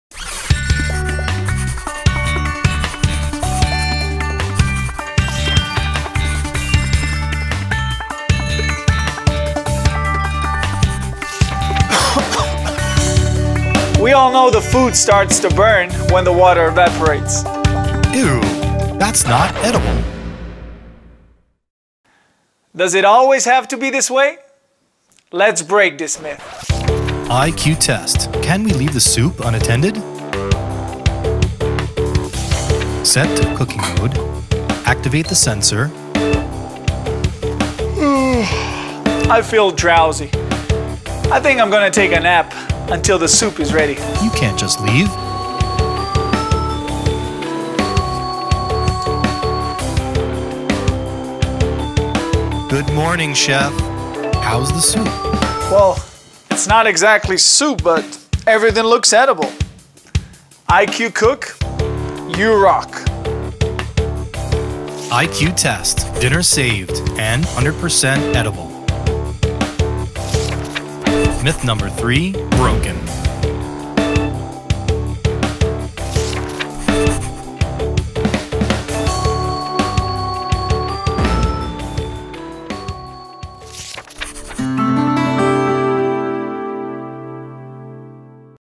Gorenje: Myth Breaking with IQcook - Burning food
Myth #3: "The food starts to burn when the water evaporates"

Let's breake this myth! (with IQcook hob)

IQ test: "Can we leave the soup unattended?"

THE GENIOUS REVOLUTION IN COOKING!